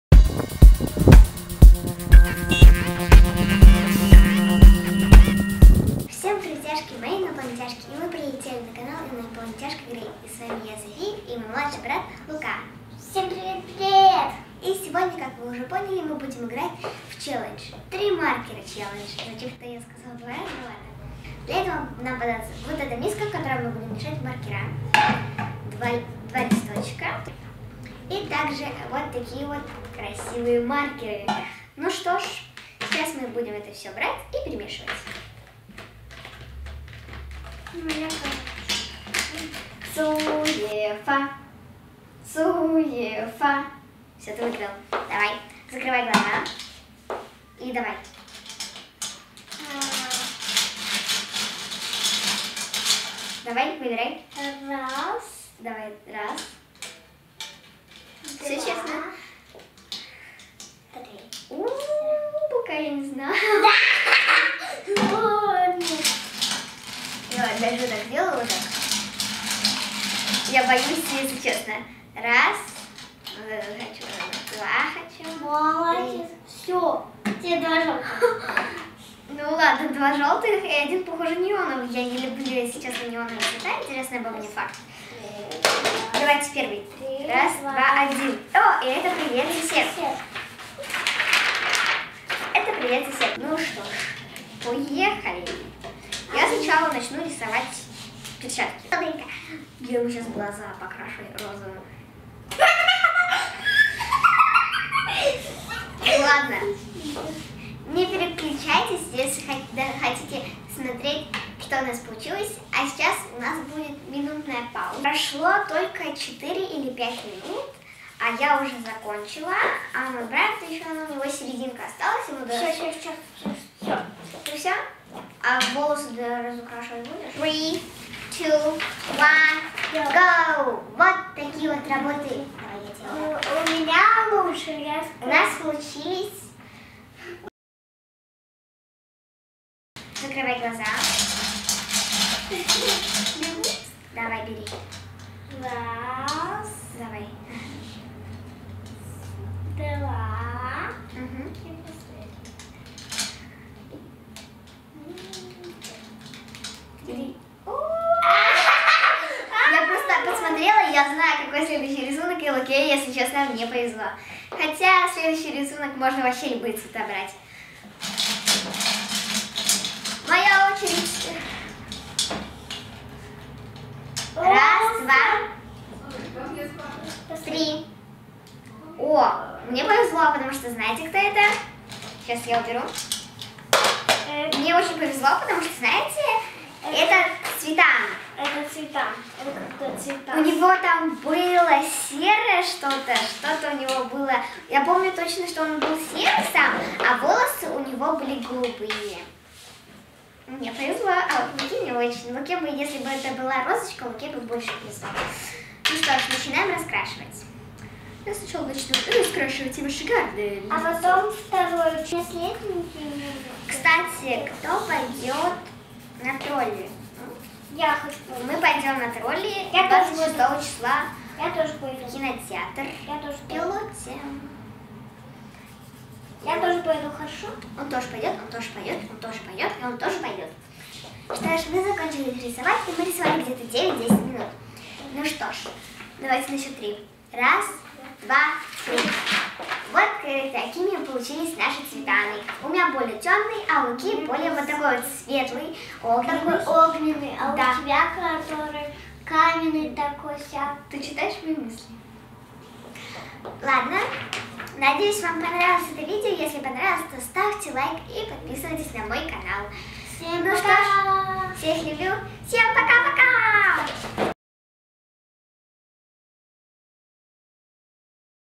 Влог-тест. Лукиян и София. Челлендж "Три маркера"
Первая работа наших детей. Даже камеру выставляли сами (не без помощи "советов экспертов" от взрослых), и монтаж делали сами (в том числе был предпринят первый шаг в освоении цветокоррекции).